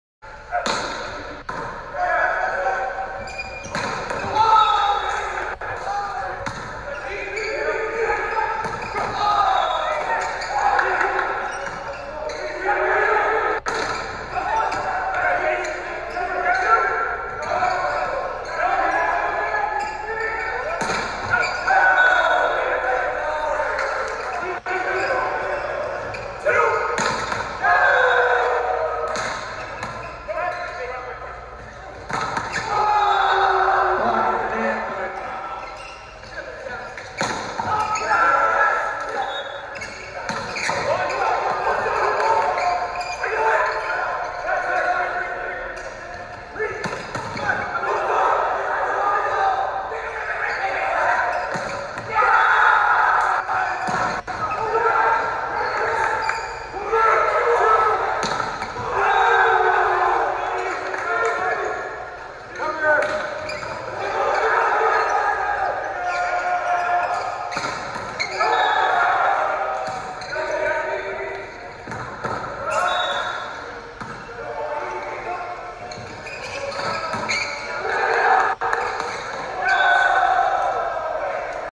#1 Hawaii Rainbow Warriors @ #3 UCSB Gauchos Volleyball 3/12/21 Short Highlights
Men's Volleyball

Hawaii sweeps Santa Barbara  -18, -21, -20